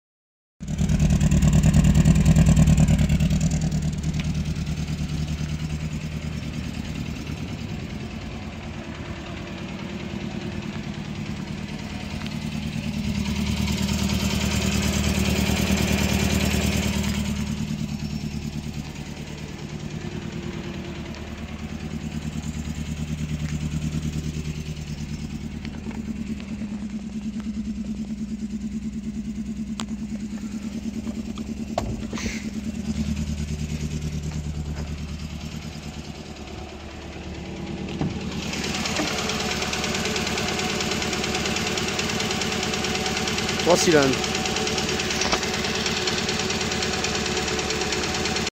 309 SR 1.6 sur 3 cylindres
Voilà le son d'une 309 SR lorsque elle tourne seulement sur 3 cylindres